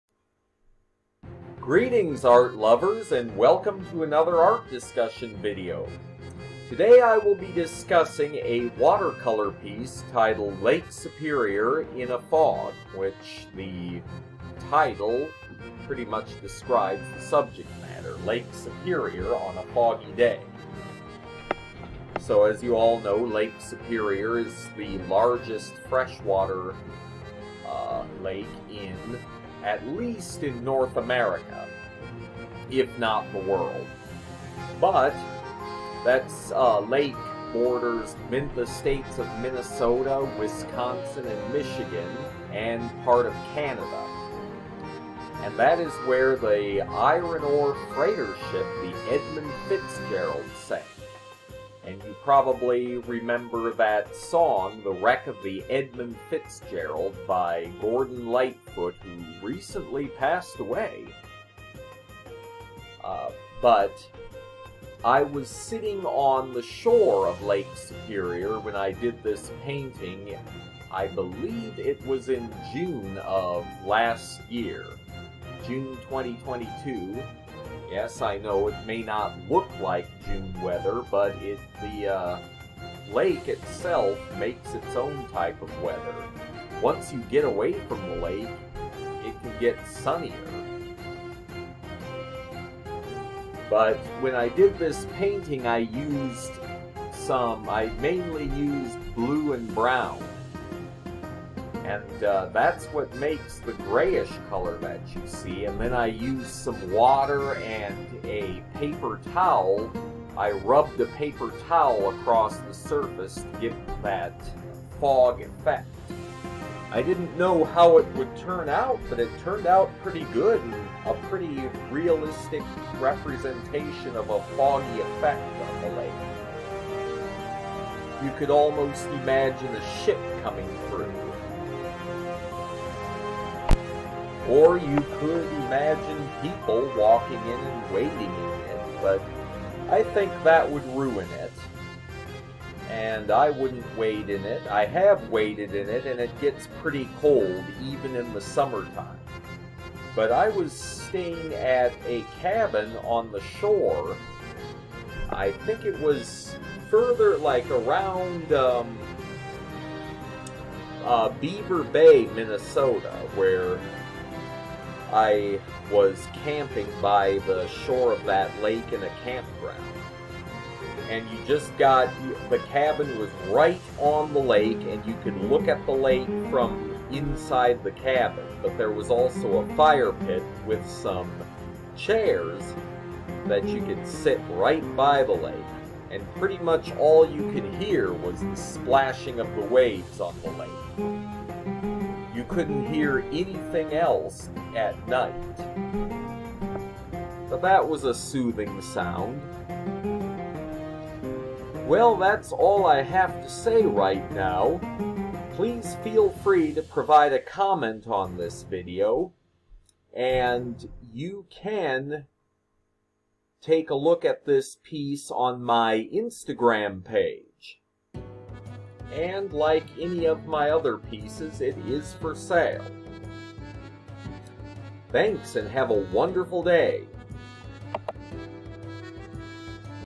Video Discussion: Lake Superior in Fog
I will discuss a watercolor I did of Lake Superior on a foggy day.